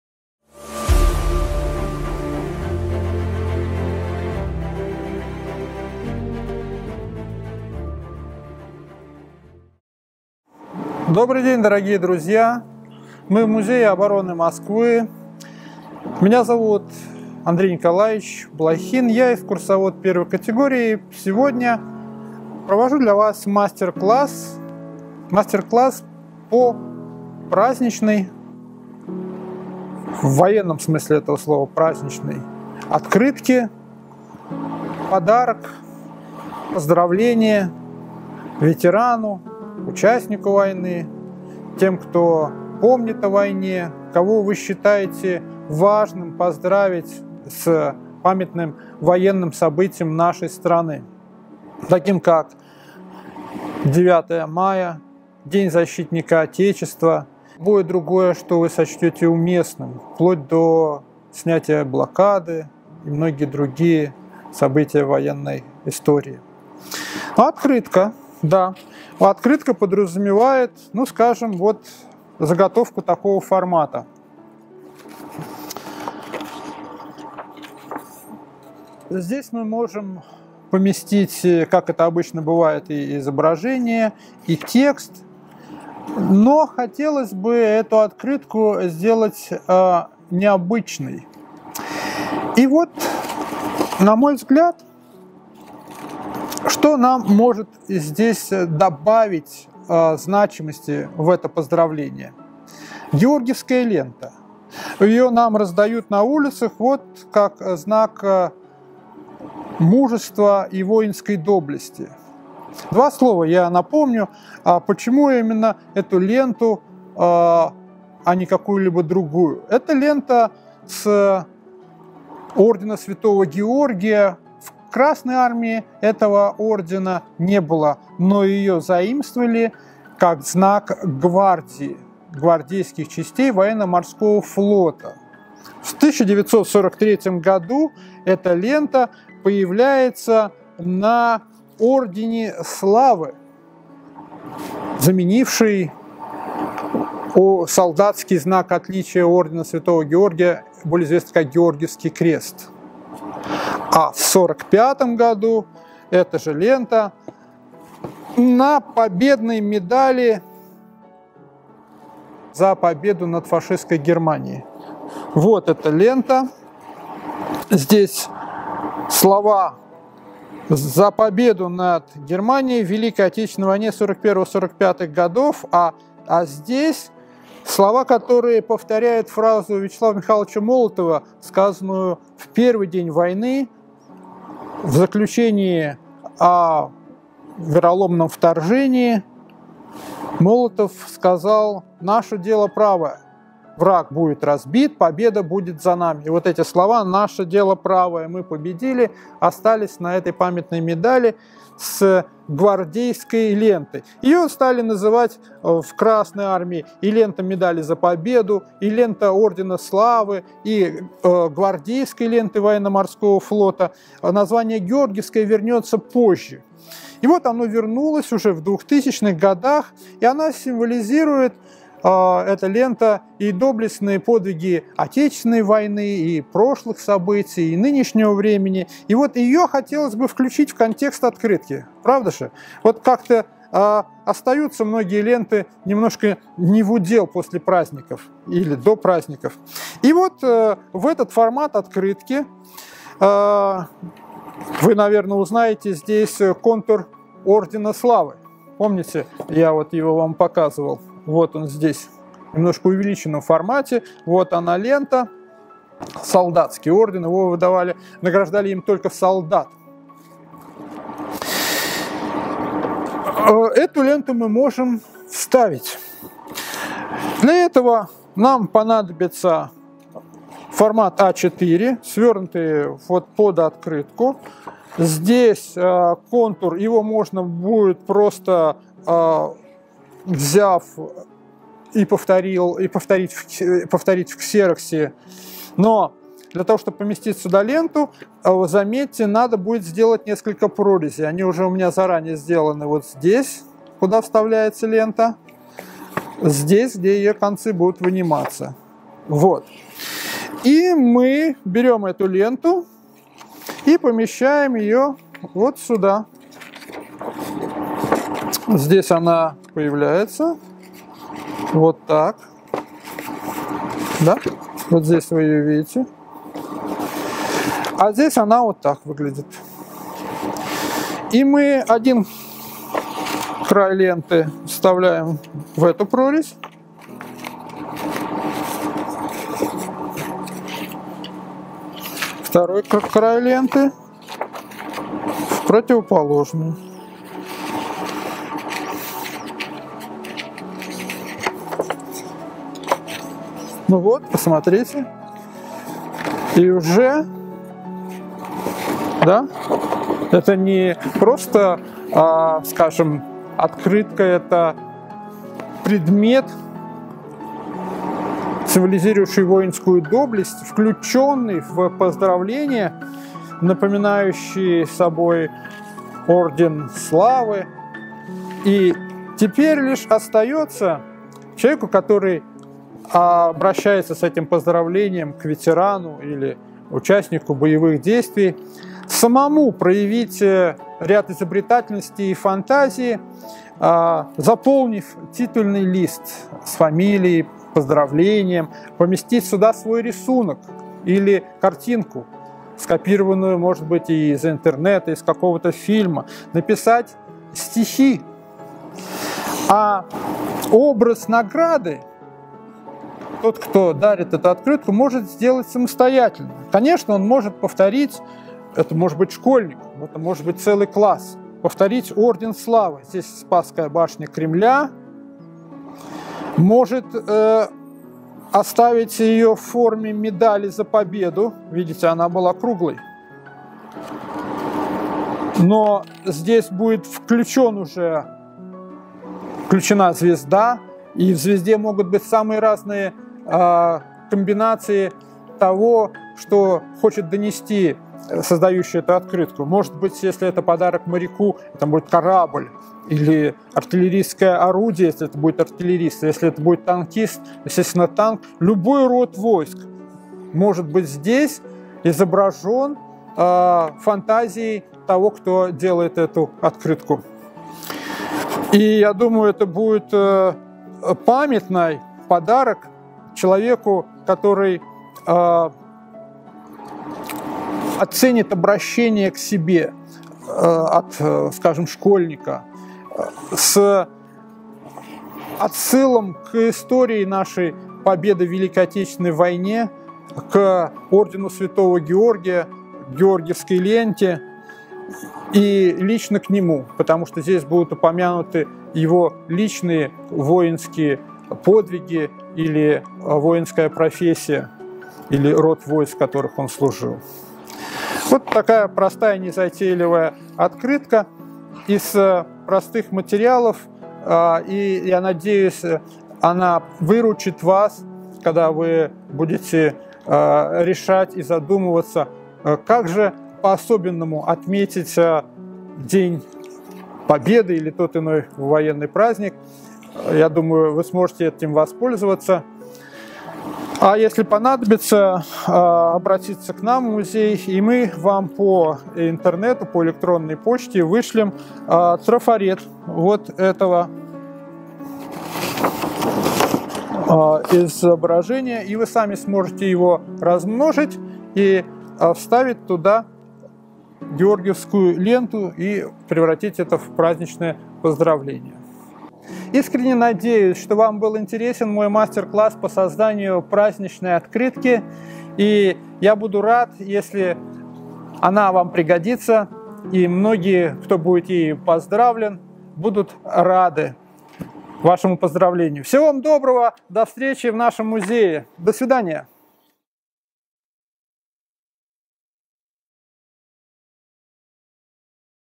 "Открытка для Героя Победы"
Государственный музей обороны Москвы продолжает серию мастер-классов. Представляем вашему вниманию мастер-класс по созданию открытки «Открытка для Героя Победы». Экскурсовод 1-й категории Андрей Николаевич Блохин расскажет и покажет, как с помощью листа бумаги, карандаша, канцелярского ножа и георгиевской ленты сделать памятный подарок Герою Победы.